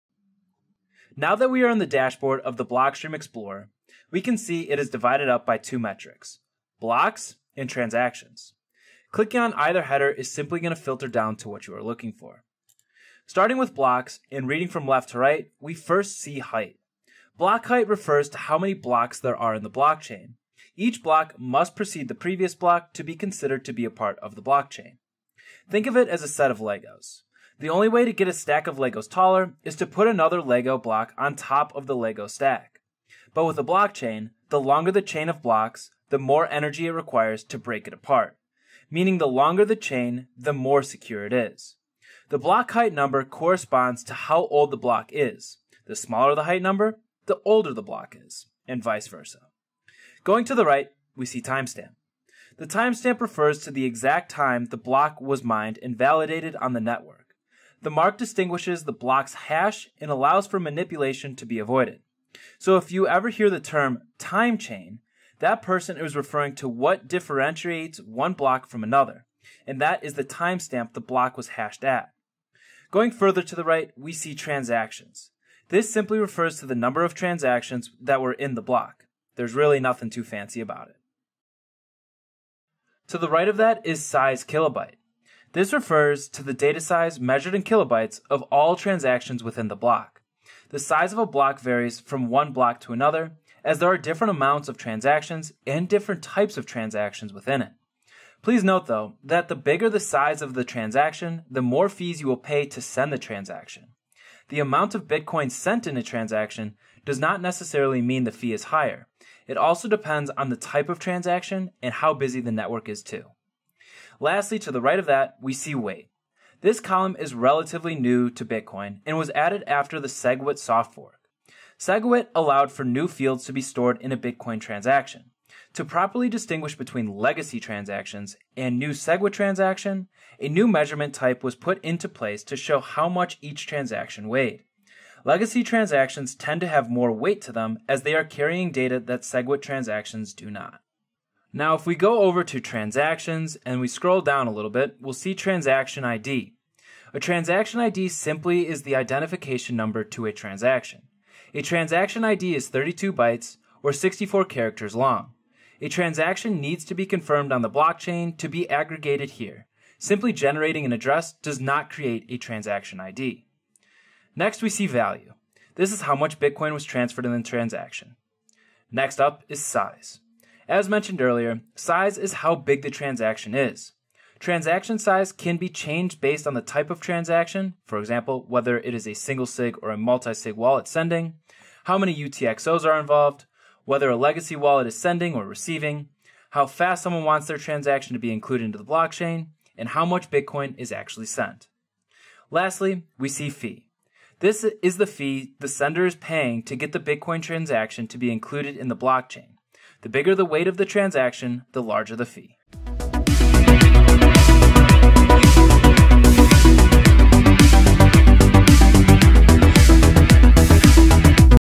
How to Read the Block Explorer
Become a Bitcoin power user by learning how to use the block explorer.

See the whole Deciphered where we do a complete rundown of blockstream.info here.